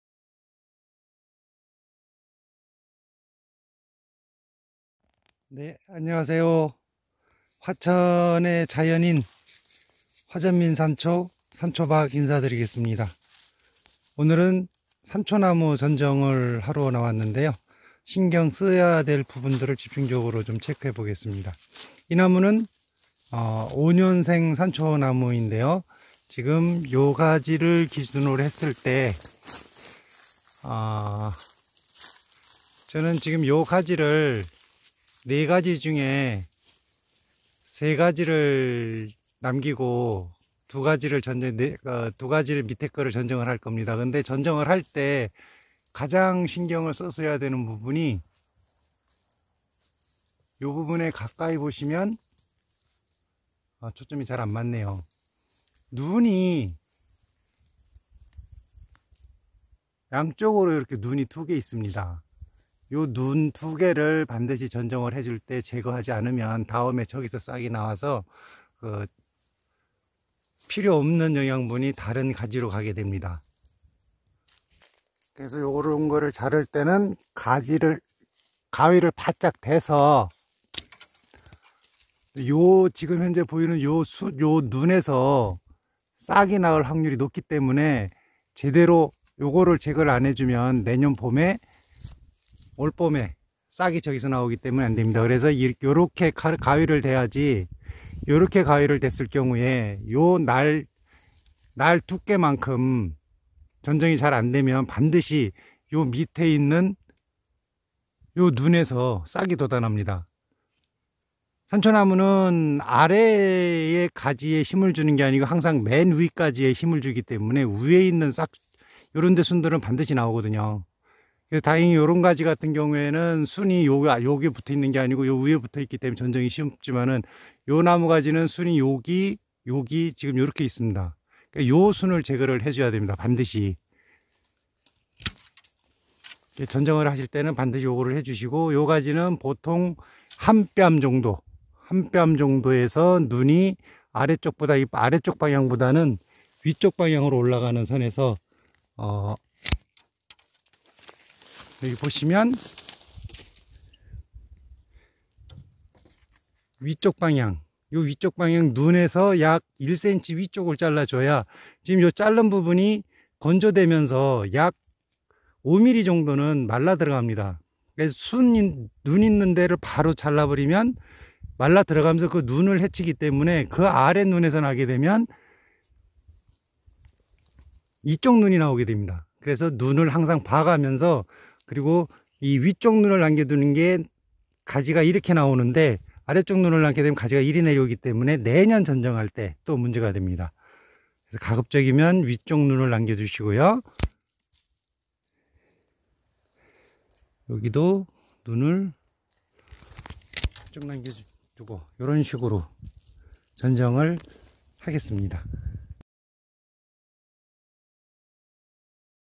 산초나무 전정 체크포인트 세가지
산초나무는 전정이 중요 합니다.
올해 나온 신초 끝에 한송이가 달리기
때문에 가지수를 적게 강전정하면
도장지로 자라 열매를 달지 못하고
너무 약전정을 하면 가지수만 많고 열매는 작은송이로 달리기 때문이지요.

팁1. 한가지 2~3가지만 남긴다
팁2. 길이는 한뼘을 기본으로
팁3. 눈을 완전 제거